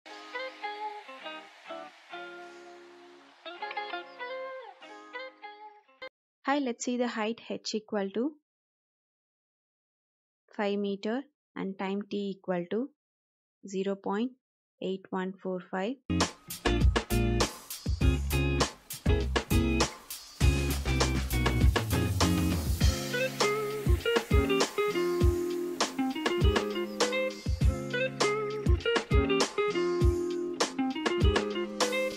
Your spaceship lands on an unknown planet. To determine the characteristics of this planet, you dro…
Your spaceship lands on an unknown planet. To determine the characteristics of this planet, you drop a wrench from 5.00 m above the ground and measure that it hits the ground 0.814 s later. What is the acceleration of gravity near the surface of this planet? Assuming that the planet has the same density as that of earth (5500 kg/m3), what is the radius of the planet?